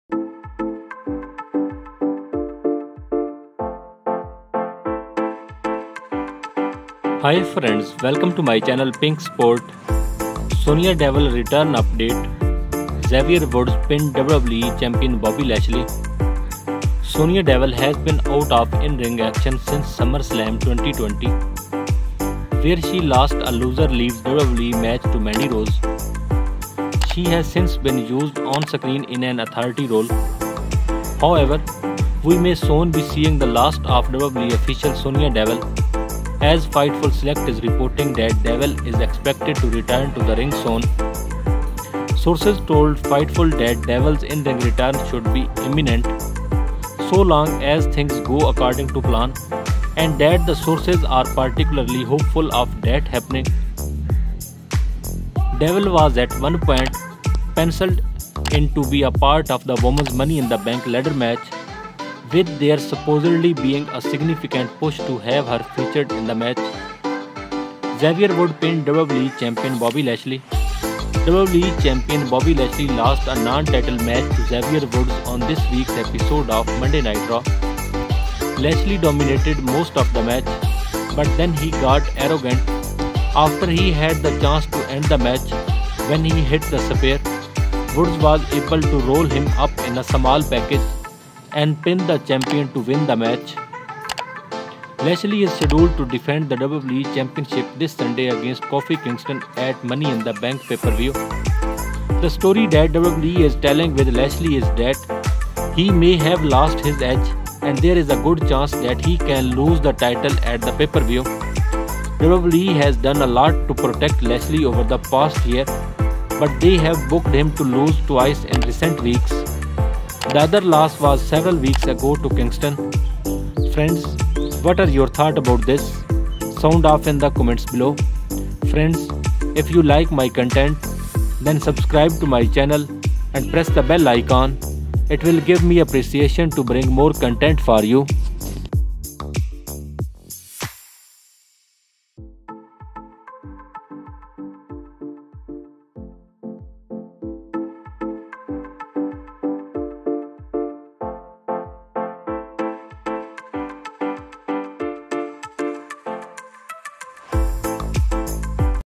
Sonya Deville Return Update - Xavier Woods Pinned WWE Champion Bobby Lashley | Pink Sport
Welcome to Pink Sport

* Hit Like
* Comment To Encourage Us